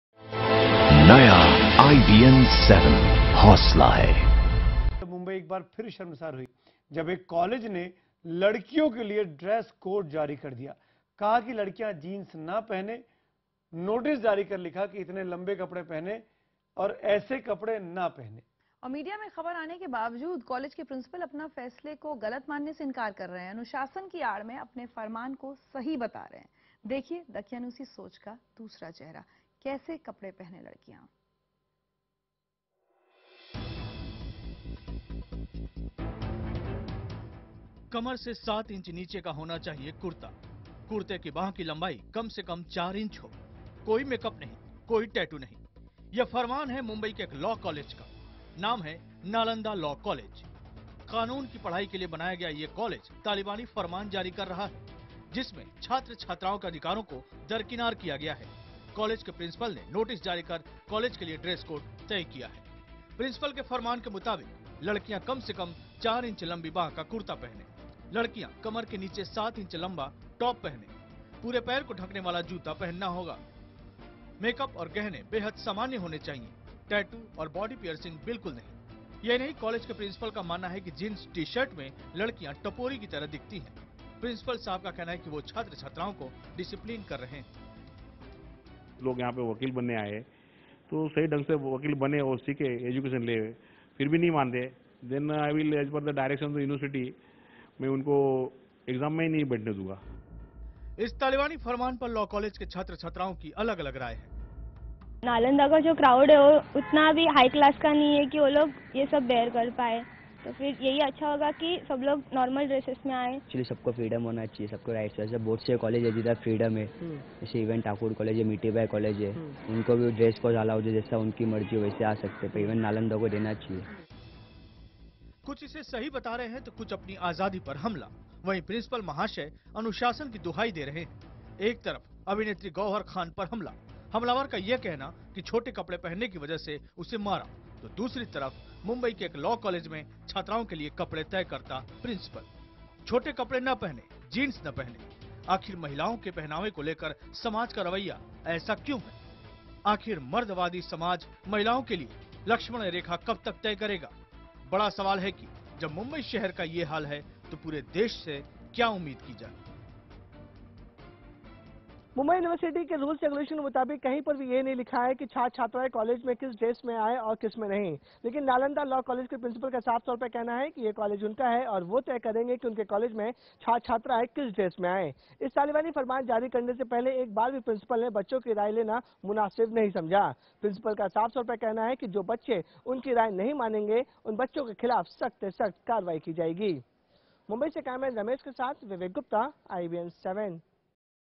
College ka ladkiyon Ke Jeans- Sandals Par Ban
मुंबई के एक कॉलेज ने लड़कियों के लिए ड्रेस कोड जारी कर दिया और कहा गया है कि लड़कियां जींस न पहनें, लंबे कपड़े पहनें।